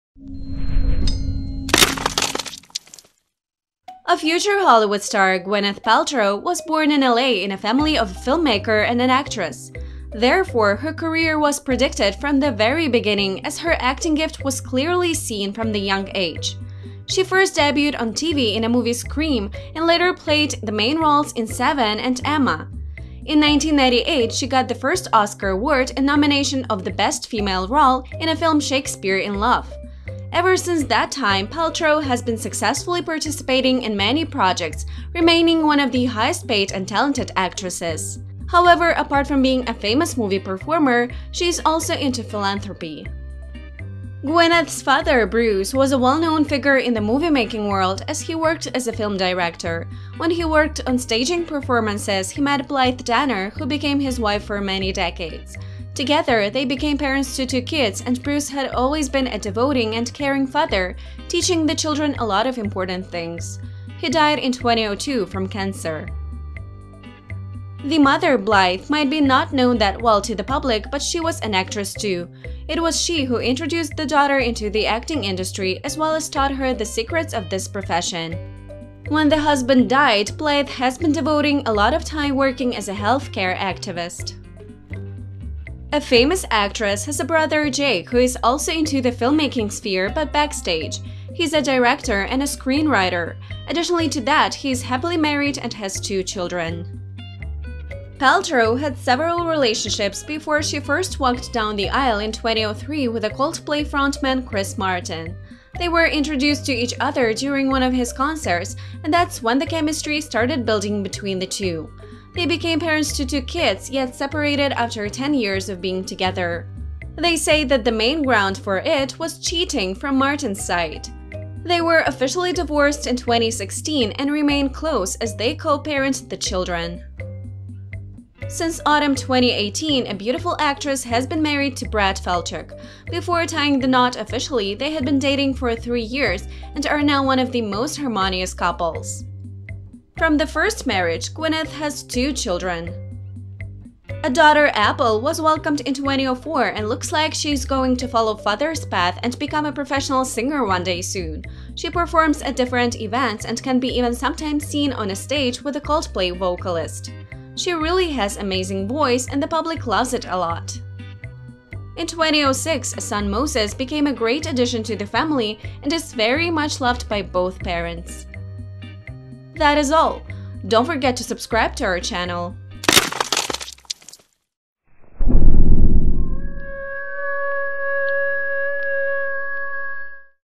Gwyneth Paltrow Family: Husband, Kids, Siblings, Parents
A future Hollywood star Gwyneth Paltrow was born in LA in a family of a film-maker and an actress. Therefore, her career was predicted from the very beginning as her acting gift was clearly seen from the young age. She first debuted on TV in a movie Scream and later played the main roles in Seven and Emma. In 1998 she got the first Oscar Award in nomination of the best female role in a film Shakespeare in Love. Ever since that time, Paltrow has been successfully participating in many projects remaining one of the highest-paid and talented actresses. However, apart from being a famous movie performer, she is also into philanthropy.

Gwyneth’s father Bruce was a well-known figure on the moviemaking world, as he worked as a film director. When he worked on staging performances, he met Blythe Danner, who became his wife for many decades. Together, they became parents to two kids and Bruce had always been a devoting and caring father teaching the children a lot of important things. He died in 2002 from cancer.

The mother Blythe might be not known that well to the public but she was an actress too. And it was she, who introduced the daughter into the acting industry as well as taught her the secrets of this profession. When the husband died, Blythe has been devoting a lot of time working as a healthcare activist.

A famous actress has a brother Jake, who is also into the filmmaking sphere but backstage. He is a director and a screenwriter. Additionally to that he is happily married and has two children.

Paltrow had several relationships before she first walked down to the aisle in 2003 with a Coldplay frontman Chris Martin. They were introduced to each other during one of his concerts and that’s when the chemistry started building between the two. They became parents to two kids yet separated after ten years of being together. They say that the main ground for it was cheating from Martin’s side. They were officially divorced in 2016 and remain close as they co-parent the children.

Since autumn 2018, a beautiful actress has been married to Brad Falchuk. Before tying the knot officially they had been dating for three years and are now one of the most harmonious couples.

From the first marriage, Gwyneth has two children.

A daughter Apple was welcomed in 2004 and looks like she is going to follow father’s path and become a professional singer one day soon. She performs at different events and can be even sometimes seen on a stage with the Coldplay vocalist. She really has amazing voice and the public loves it a lot.

In 2006 a son Moses became a great addition to the family and is very much loved by both parents.